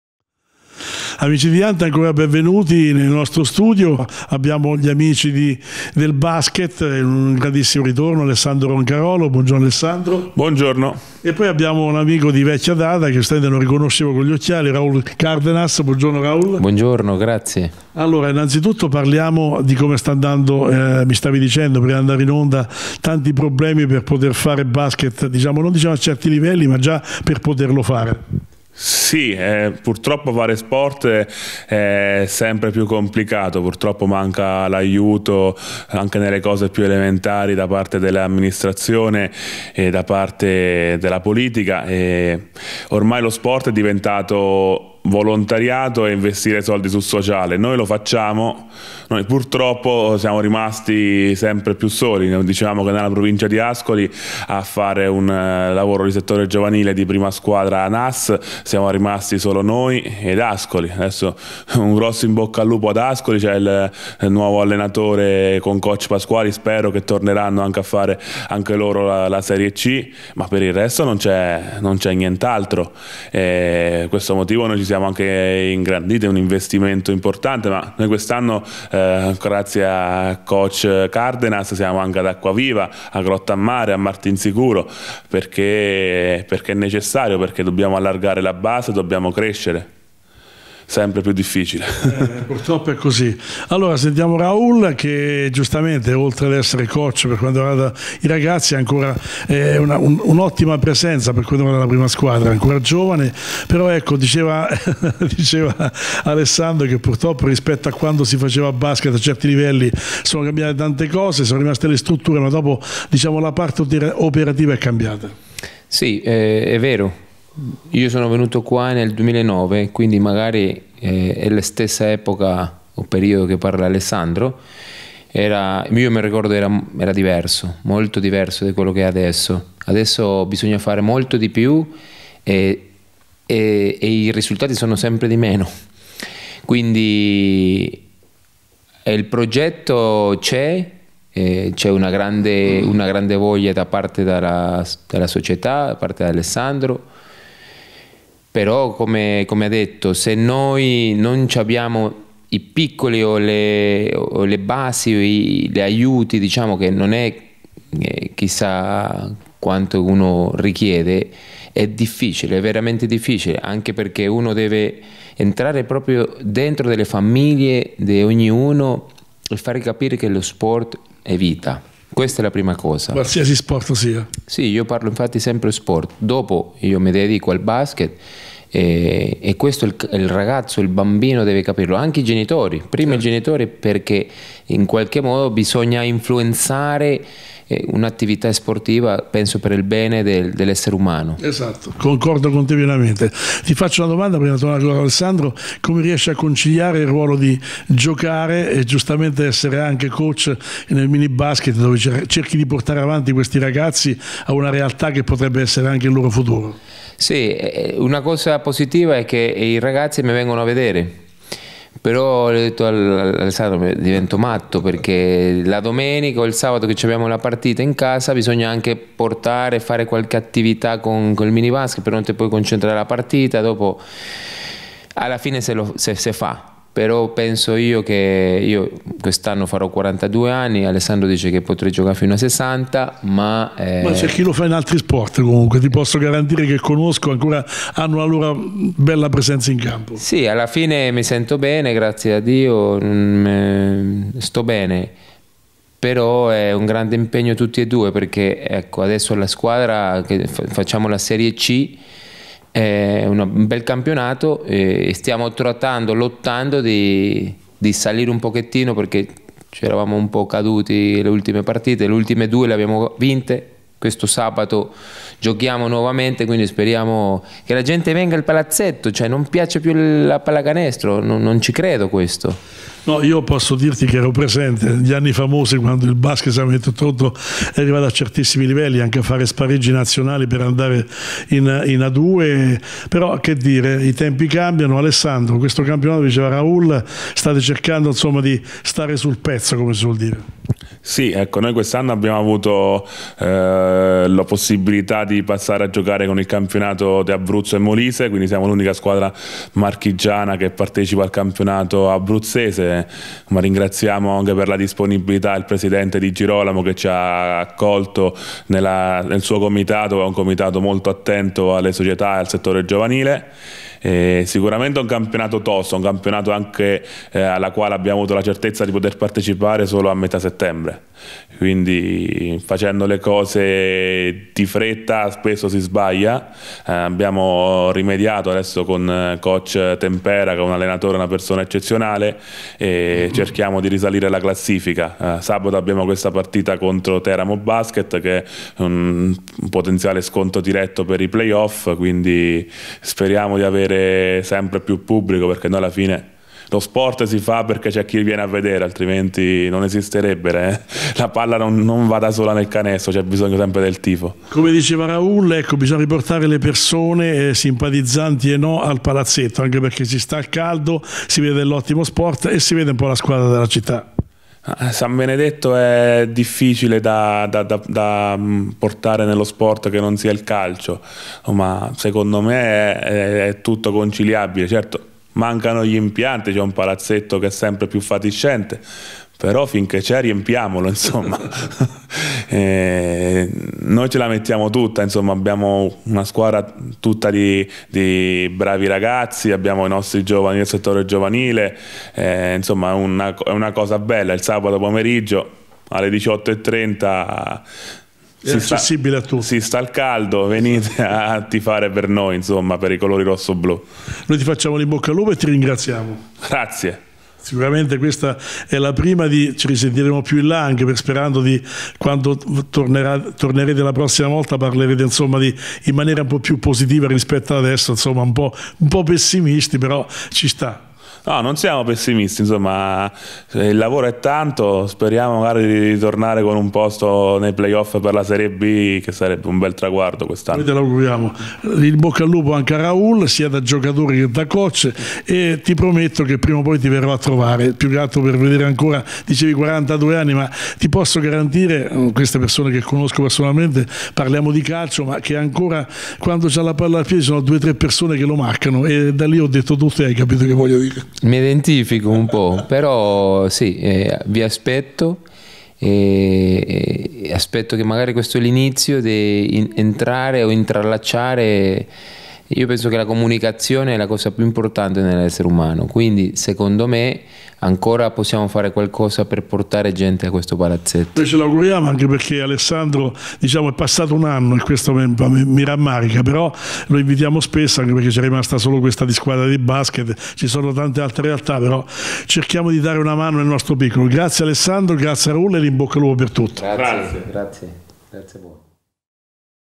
Jant.it | Ospite nei nostri studi, la Sambenedettese Basket
In occasione dell'incontro del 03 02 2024 alle 18.30 presso il Palazzetto dello Sport di San Benedetto del Tronto abbiamo parlato delle attività sportive che la società cestistica sta portando avanti.